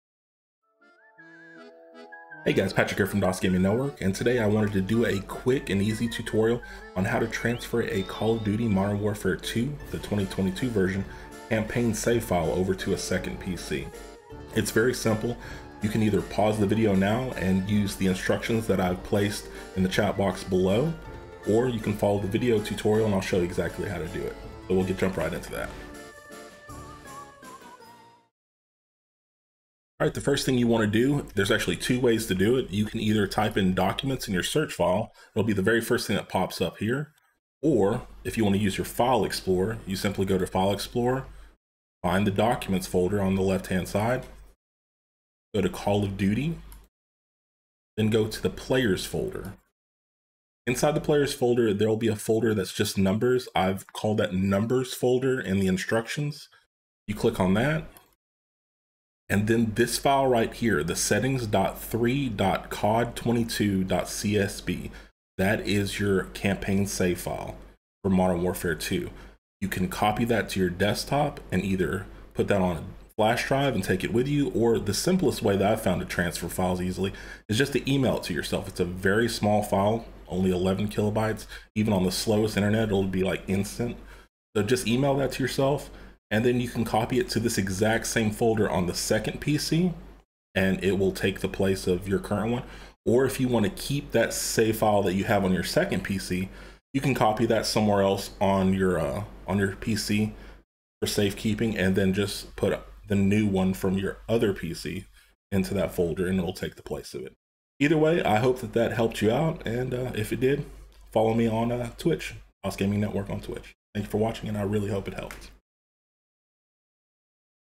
MWII (2022) Save File Transfer
How to transfer Modern Warfare II campaign save file to different PC. Works for Battle.net and Steam. If this helped you please check out my Twitch @ I stream pretty much every night around 7:30pm 🎃